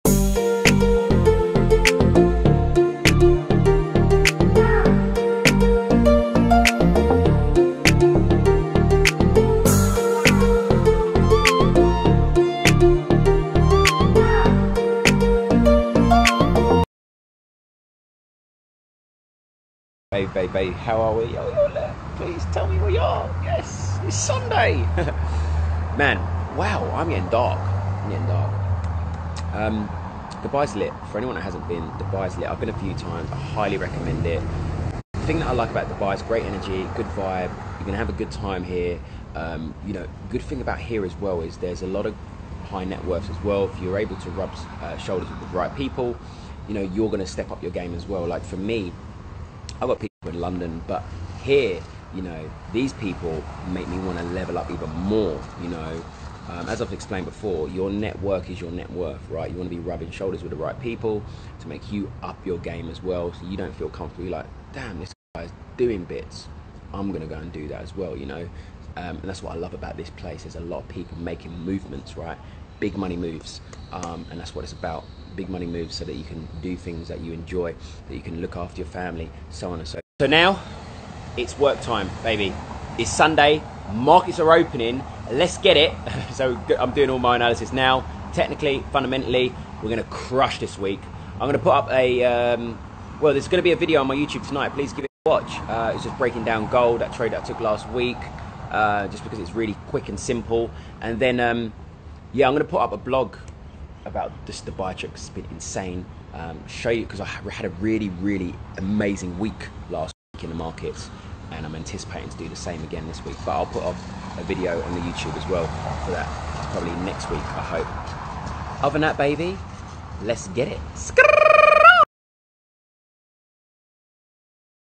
KenFx Freak talks the Leveling Up that comes with being in Dubai 🚀💰
Ken Chigbo talks the Levelling Up that comes with being in the UAE. Do you agree with him, Top Traders?